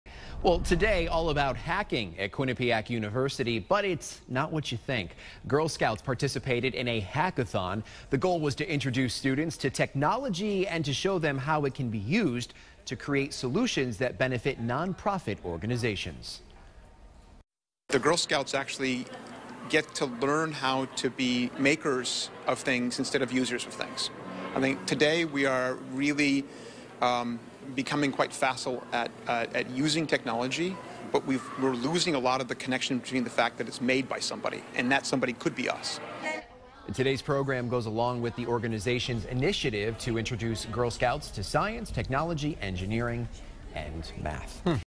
Girl Scouts participate in "Hackathon" at Quinnipiac University
It was all about hacking at Quinnipiac University on Saturday, but it's not what you think.